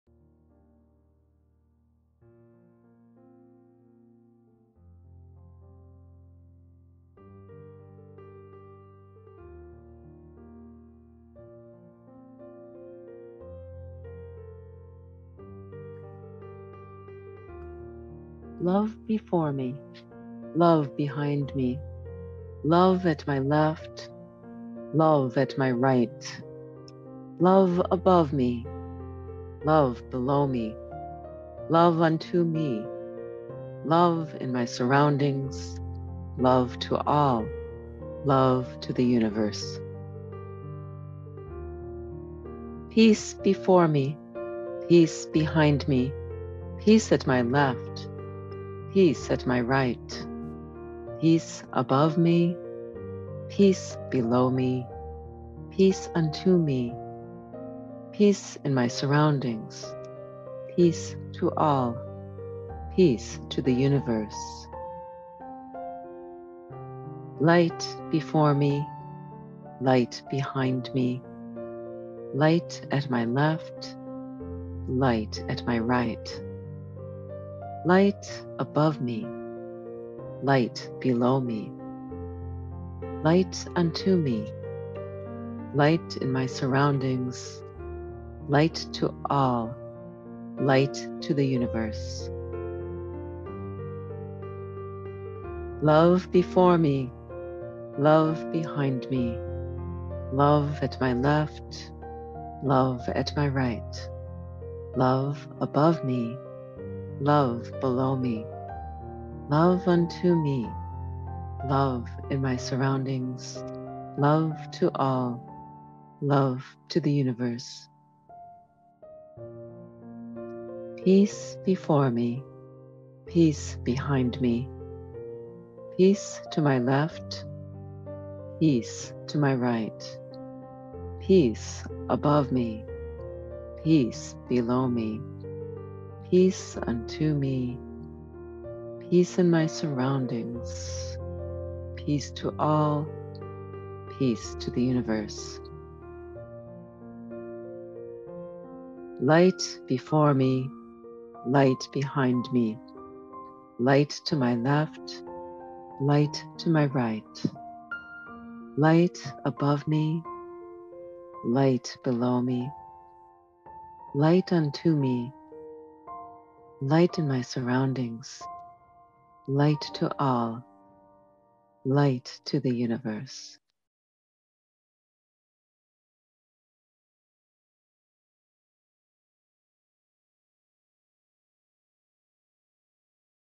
Prayer for World Peace and Protection (Prayer of Love Peace and Light)
At this juncture of collective awakening many are calling for prayer for world peace. While much is happening in the outer world, it is critical to realize that peace begins from within.

I recently discovered this beautiful prayer attributed to the founder of Naam Yoga, Dr. Joseph Michael Levry. When used as a mantra and focusing your attention on the heart, The Prayer of Love Peace and Light creates a powerful protective field around you while at the same time sending blessings out to the universe.

The Prayer of Love Peace and Light is an affirmation prayer that activates the highest energies of light in you. It focuses your awareness on heart qualities, raises your vibration, and seals your aura from all 7 directions, magnetizing your field to attract higher frequencies of Light and repel lower dimensional energies. Then, it sends out concentrated heart energy to all around you and to the ends of the universe. As a prayer for world peace I can think of nothing more powerful.

The effects of this prayer for world peace and protection will get stronger the more often you repeat it. For maximum effect use it as a mantra often throughout the day. Allow yourself to really feel the vibrations of love, peace, and light as you speak the prayer. Feel yourself surrounded by love and light on all sides, and feel that love radiating out from your heart center as you speak (mentally or aloud) the last two lines of each verse.

Please remember that much needs to be dissolved and broken down in the world right now to make room for the new, higher-consciousness beings and infrastructure that want to come into being. It is up to each one of us to consciously embody the peace that we want to see in the world, no matter what appears to be happening around us. In this way we create the world we envision- one breath and one prayer at a time.

(The art in the video is my own; visit my gallery site to view more

🐢 Spirit Animal Awareness Oracle Card Deck